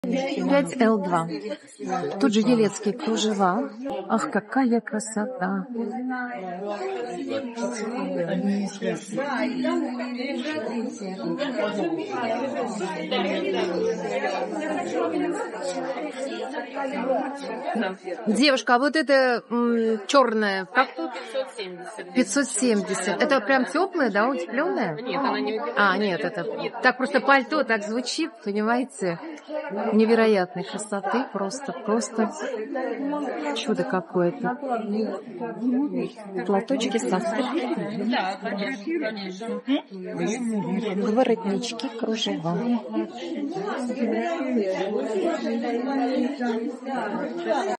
Выставка-ярмарка «жар-птица. Осень 2024»
«Елецкие кружева» на XXIII выставке-ярмарке народных художественных промыслов и ремесел России «Жар-птица. ОСЕНЬ 2024».